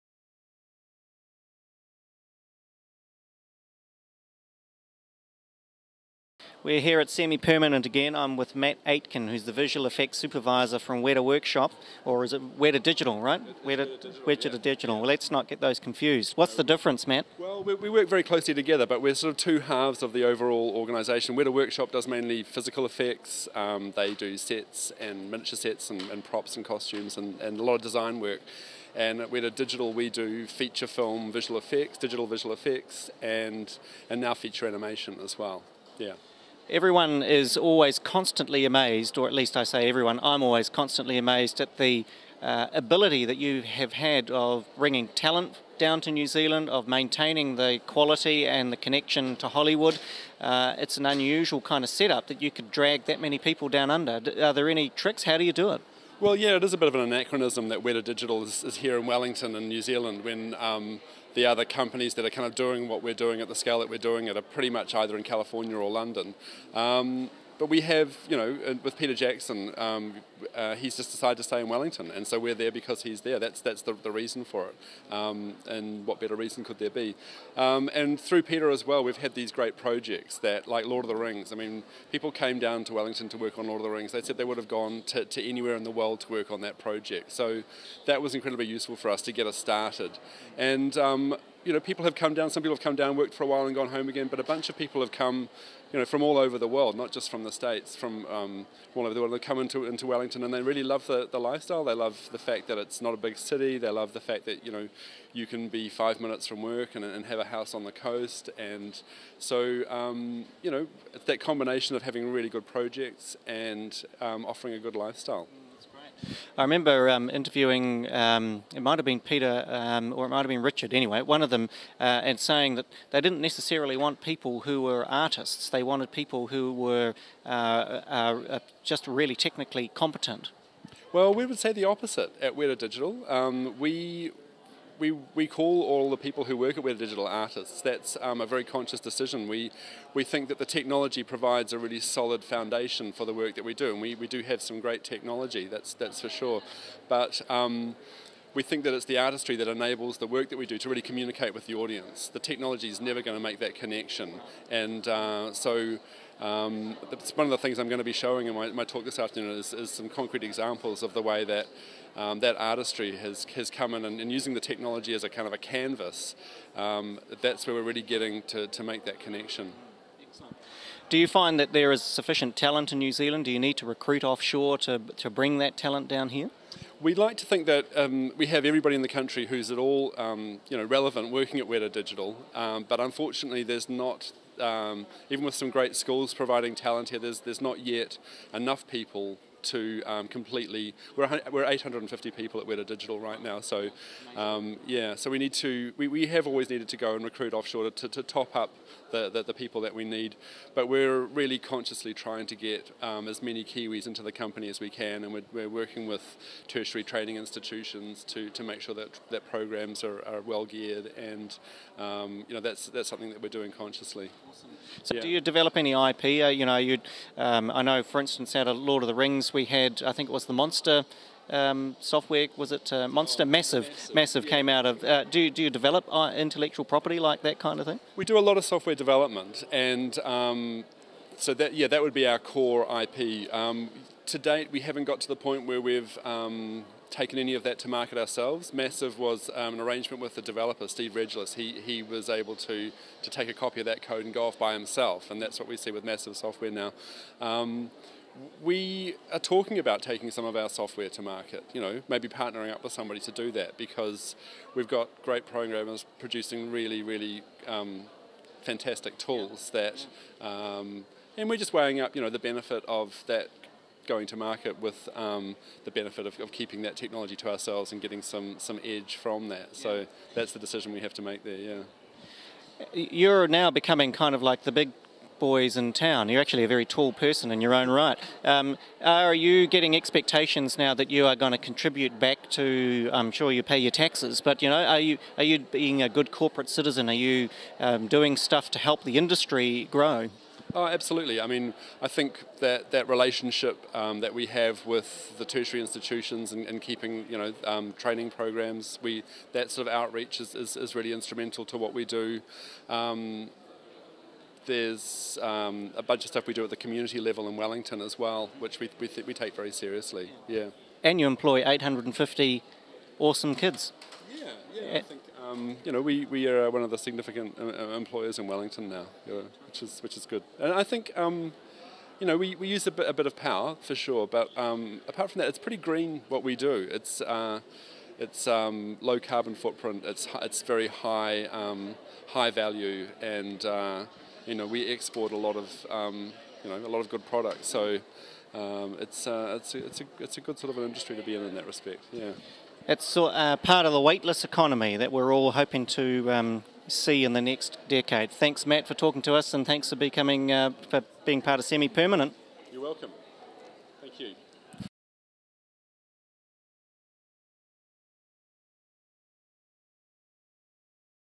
Giant Weta - Matt Aitken at Semi-Permanent Auckland 09
He's very tall and filled to the brim with creative prowess. Weta Digitals visual effects supervisor Matt Aitken tells Vincent Heeringa why Hollywood loves New Zealand.